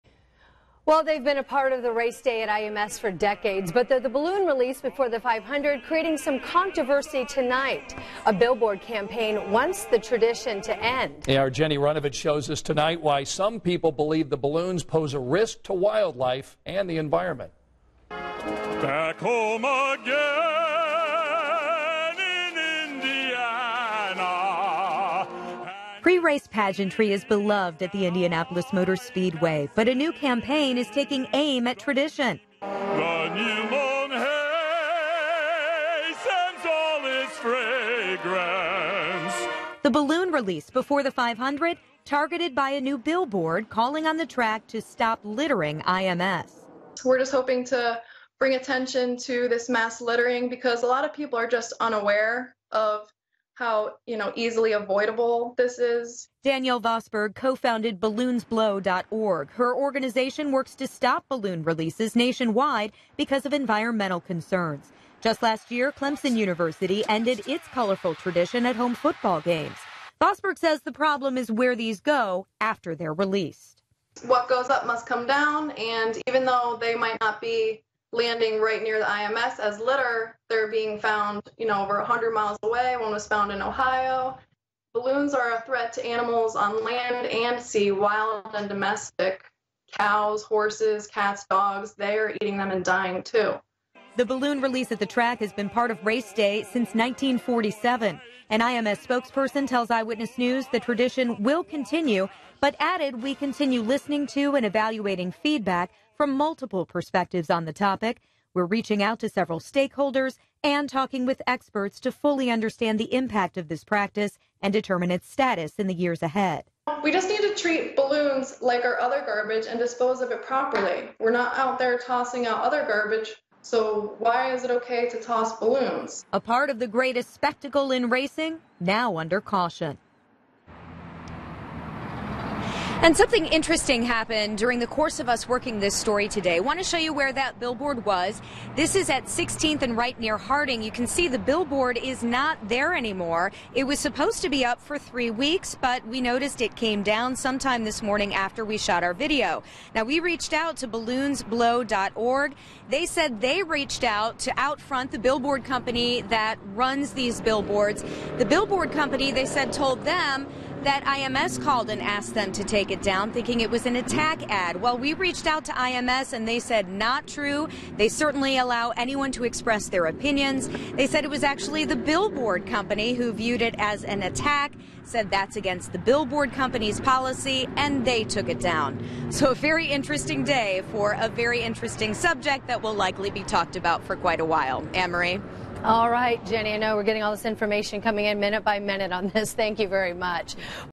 Battle over Indy 500 balloons
The balloon release before the Indy 500 is being targeted by a new billboard, calling on the track to "stop littering IMS".